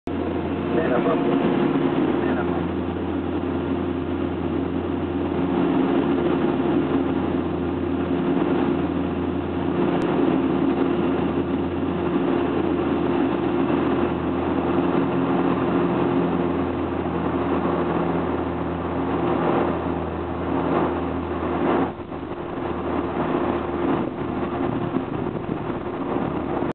Final y aterrizaje en RWY 26 SULS Punta del Este. FAU
DIA TURBULENTO, ATERRIZAJE EN RWY 26 DE LAGUNA DEL SAUCE, PUNTA DEL ESTE EN EMB120.